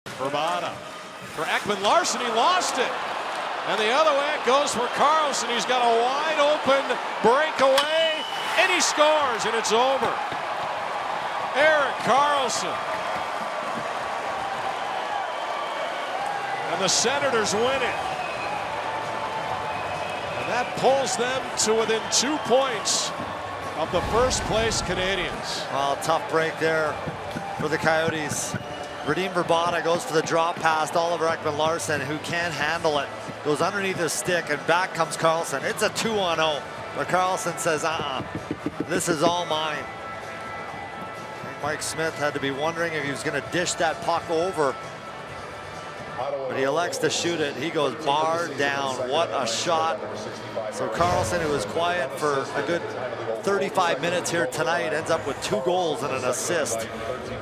Gotta See It: Karlsson given lots of time and space, snipes on Smith in overtime
Ottawa Senators captain Erik Karlsson has all the time in the world, and no one in front of him except the goalie and makes no mistake to win it for his team in overtime.